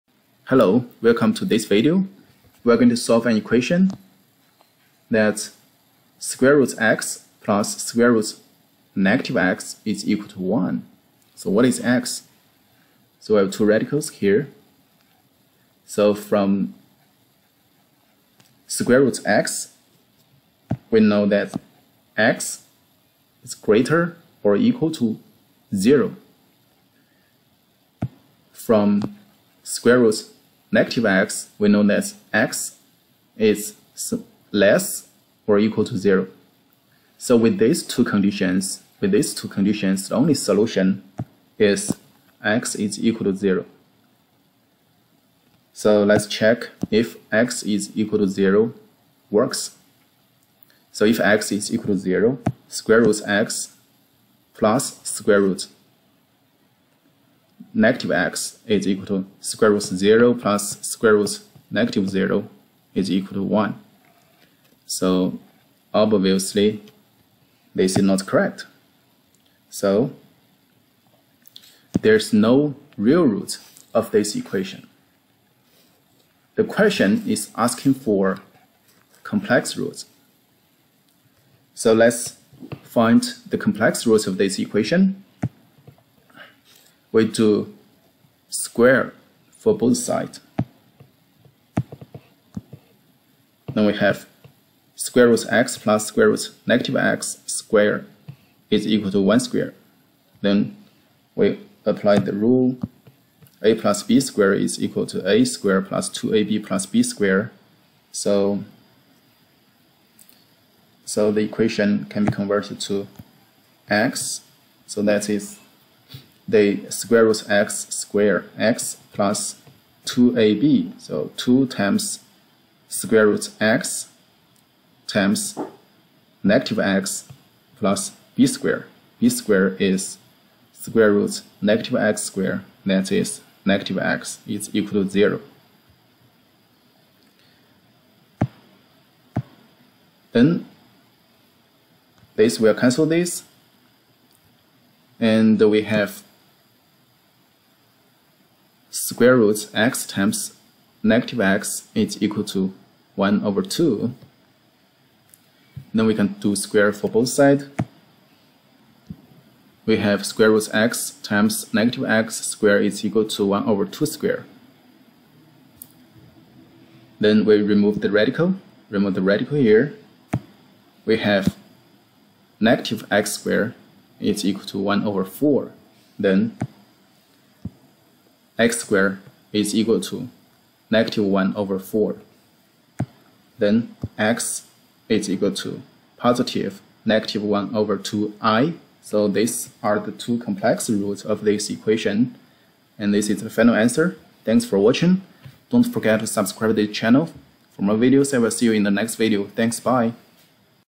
A Nice Equation | Algebra Question | Can You Find Toots of This Equation?
How to solve this equation? By using this trick, you can solve it easily!